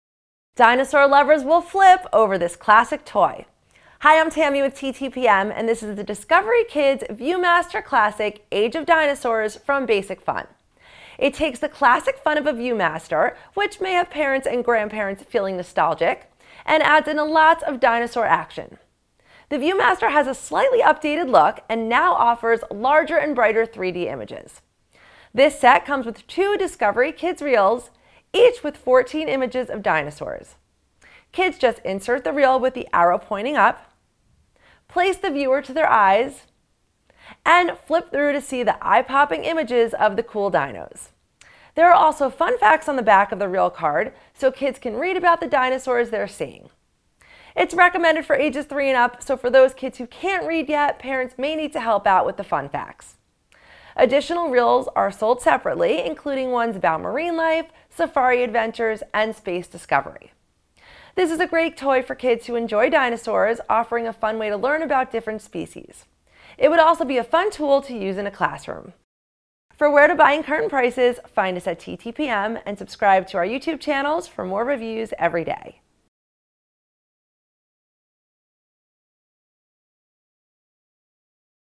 3D Discovery Kids View Master Classic Age of Dinosaurs from Basic Fun
Discovery Kids View-Master Classic Age of Dinosaurs! See 3D images of dinosaur species and read fun facts. Includes viewmaster and 2 Discovery Kids reels! 28 dinosaur images! For 3+! Classic fun for dinosaur lovers! TTPM reviews this cool toy.

Product Info:
The Discovery Kids View-Master Classic Age of Dinosaurs from Basic Fun takes the classic fun of a view master, which may have parents and grandparents feeling nostalgic, and adds in dinosaur action. The Viewmaster has a slightly updated look, and now offers larger, and brighter 3D images. This set comes with two Discovery Kids reels, each with 14 images of dinosaurs. Kids just insert the reel with the arrow pointing up, place the viewer to their eyes, and flip through to see the eye popping images of the cool dinos. There are also fun facts on the back of the reel card so kids can read about the dinosaurs they're seeing. Additional reels are sold separately including ones about Marine Life, Safari Adventures, and Space Discovery.